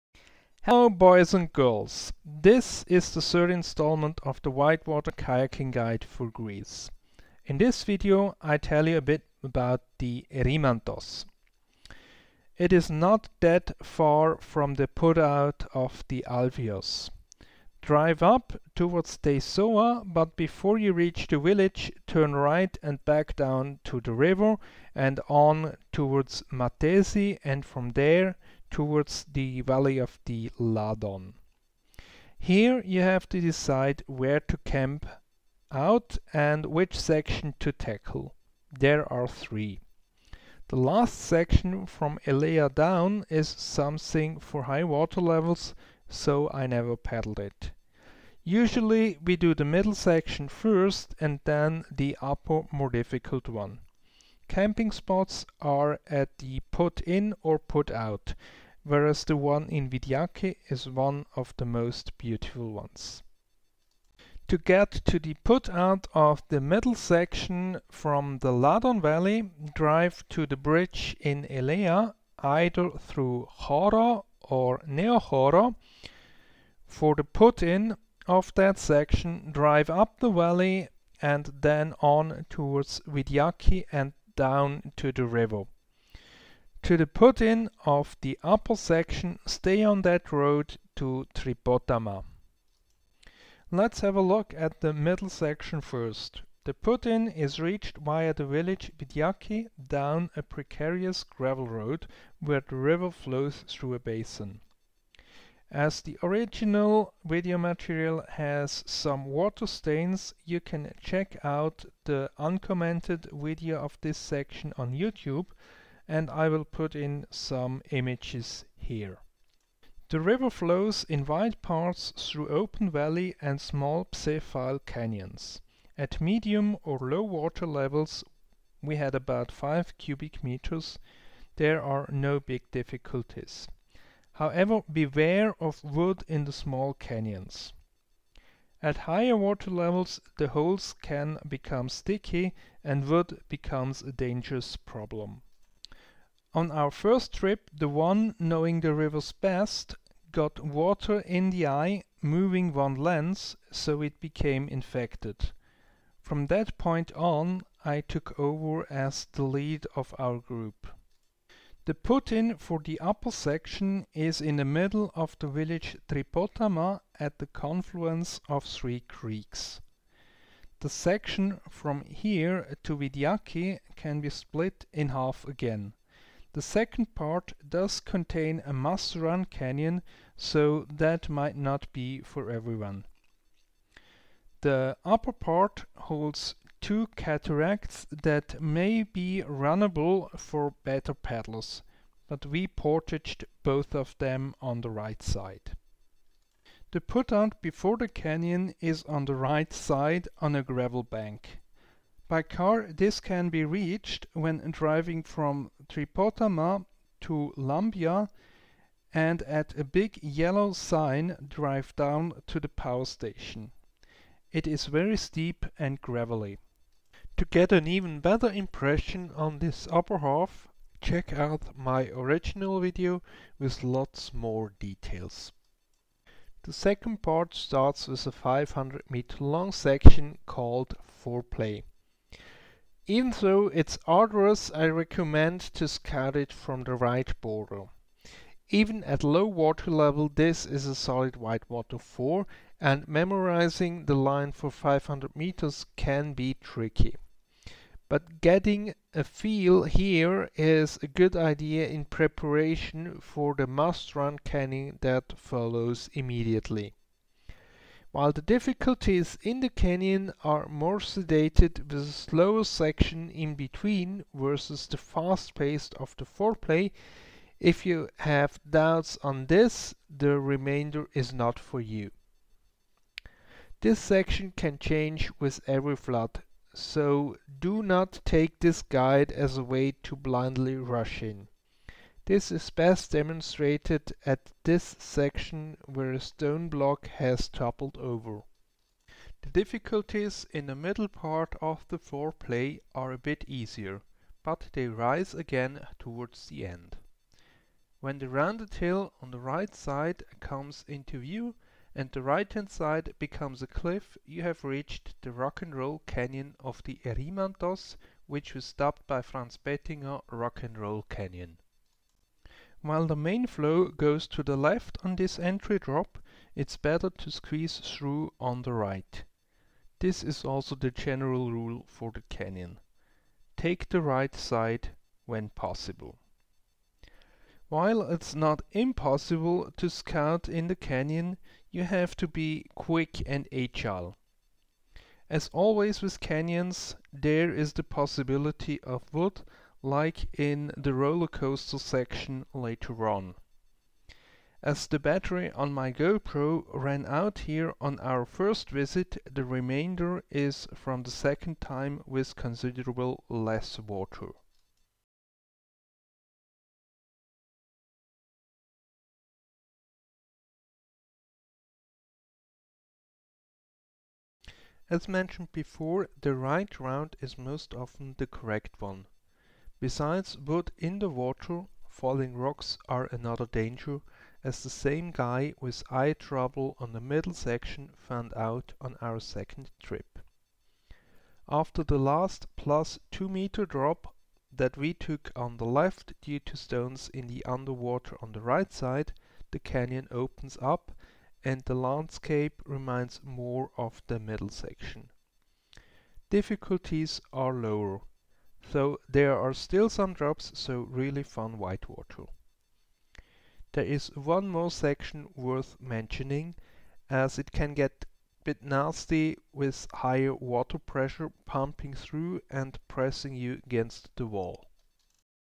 Whitewater Kayaking in Greece: A Guide - Part III: Erymanthos
Guide for kayaking in Greece. Due to the Corona situation our plans for a trip to Greece have been cancelled, which means no new material for some clips. Instead I revisit the existing videos and guide you through it with a commentary that is also available in German.
The third part covers the the middle and upper section of the Erymanthos river on Peleponese.

Put in coordinates Vidiaki: N37.7782 / E21.7961
Put out coordinates Elea: N37.7008 / E21.8087
Put in coordinates Tripotama: N37.8675 / E21.8892
Put out at power station: N37.82944 / E21.82353
Put out coordinates Vidiaki: N37.7782 / E21.7961